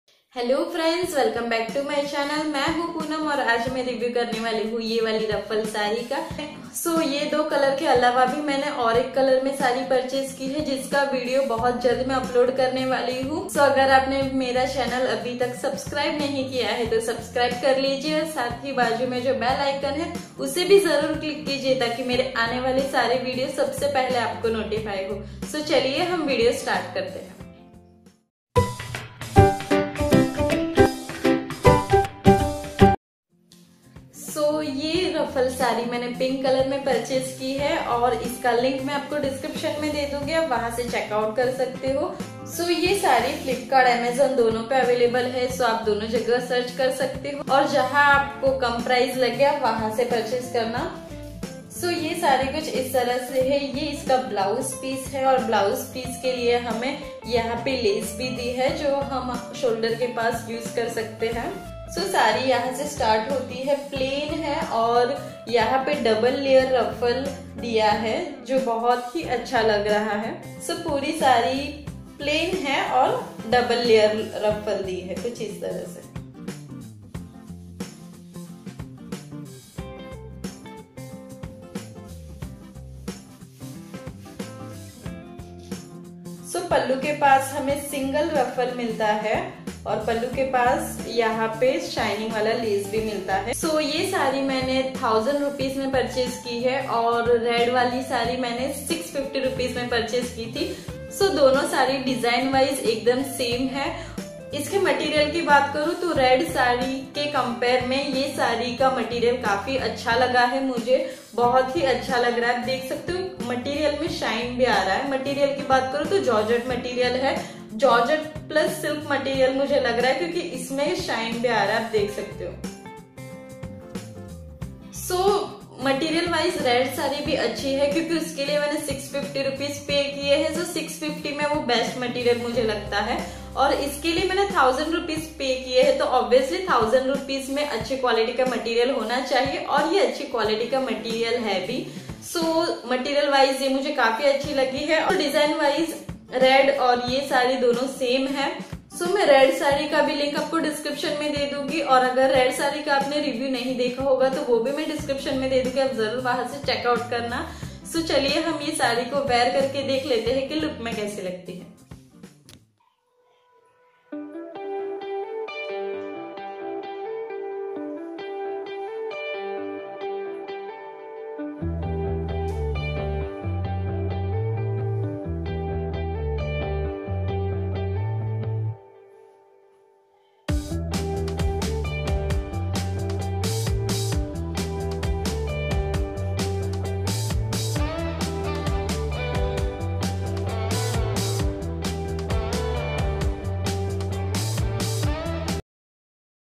Amazon Ruffle saree review
buying link
Magneitta Women's Georgette Solid Ruffle Saree (Pink)

buying link
Jaanvi Fashion Women's Ruffle Georgette Saree (frill-saree-ruffle-red)